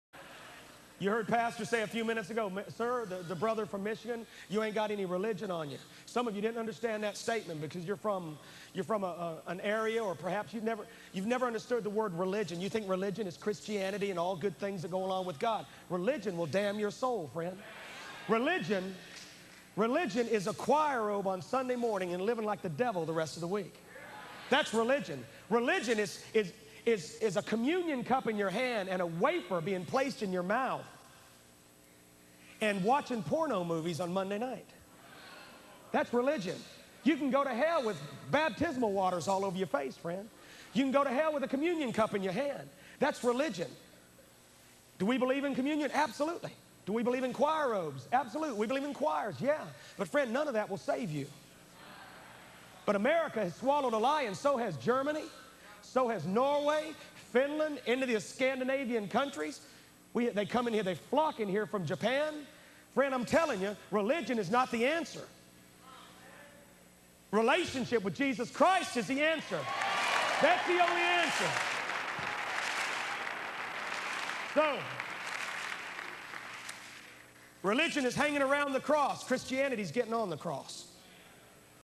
Steve Hill - Religion vs. Relationship
You can go to Hell with a choir robe on, baptismal waters on your face, and a communion cup in your hand. Religion is hanging around the cross. Christianity is getting on the cross. Religion will damn you to Hell. Do you have a relationship with Jesus Christ?